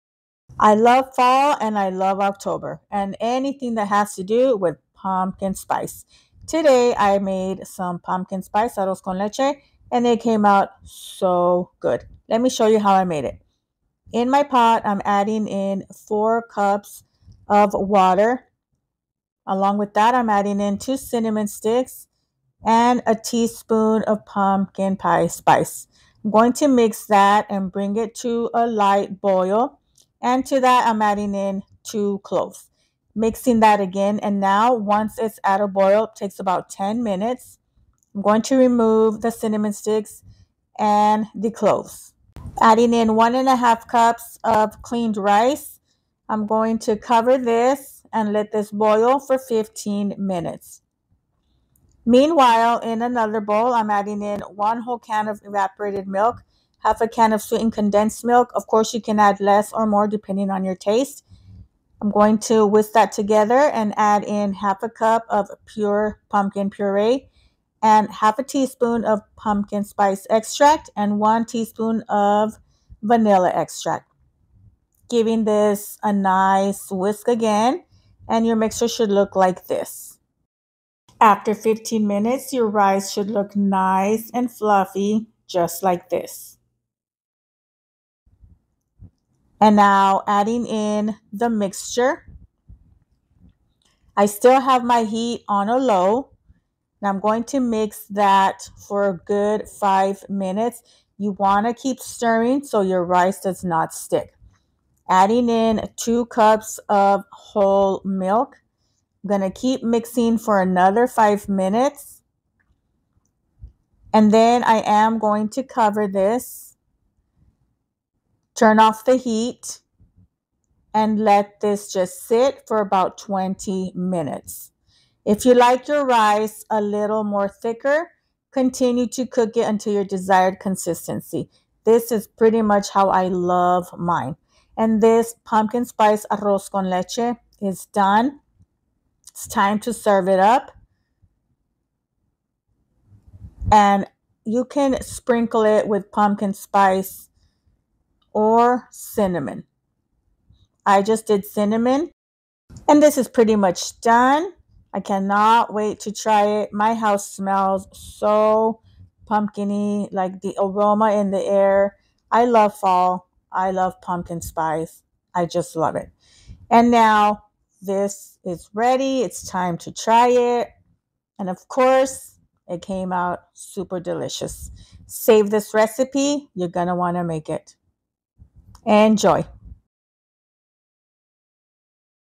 🎃Pumpkin Spice Arroz con Leche. Perfect for Fall & October
Pumpkin Spice Arroz con Leche is so creamy and delicious. This is where Mexican desserts meet your favorite fall foods. An easy-to-make sweet treat for the entire family. I love Fall & October & anything that has to do with pumpkin spice. The aroma & taste just makes me want to dance. I know you will love this delicious treat too!

Recipe here:👇
4 cups water
2 cinnamon sticks
2 cloves
11/2 cups rice(rinsed)
1 can evaporated milk
1/2 can sweetened condensed milk
1/2 cup pure pumpkin puree
1/2 tsp pumpkin spice extract
1 tsp vanilla extract
2 cups whole milk

In a pot add the 4 cups water along with the cinnamon sticks & pumpkin pie spice. Whisk & add the cloves & slightly whisk again & with your heat on medium low cover & let boil 10 min
After 10 min remove the cinnamon sticks & the cloves & add in the rice. Stir, cover & let boil 15 minutes until rice is fluffy. Meanwhile in a bowl add in the evaporated milk,sweetened condensed milk & whisk then add the pumpkin puree,pumpkin spice & vanilla extracts & whisk
After your rice has boiled & is nice & fluffy add in the mixture & keeping the heat on low mix for 5 min & keep mixing so the rice doesn't stick
Now add in 2 cups whole milk & keep mixing for another 5 minutes
Turn the stove off,cover the pot & let sit for 20 minutes
If you like your arroz con leche thicker then just keep cooking until your desired consistency
Serve,sprinkle with pumpkin pie spice or cinnamon & enjoy😋

✅Rice should be rinsed from dirt

✅Add sweetened condensed milk to your liking,but can also substitute for sugar

✅Use pure pumpkin puree & not the pie filling

✅If you like thicker arroz con leche just keep cooking until your desired consistency

I would like to thank you all who follow me for the support & hope to share many more recipes for years to come
Much love from my kitchen to yours❤💫
Follow me on: